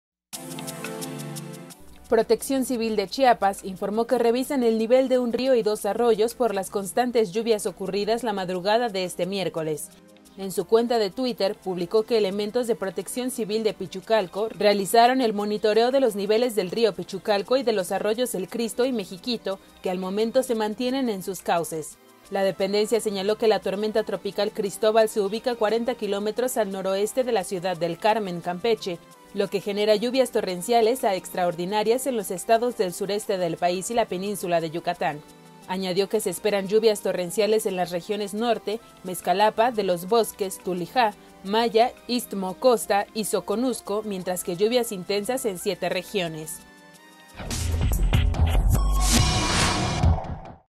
Revisan niveles de río y arroyos en Chiapas por lluvias
Protección Civil de Chiapas informó que revisan el nivel de un río y dos arroyos, por las constantes lluvias ocurridas la madrugada de este miércoles.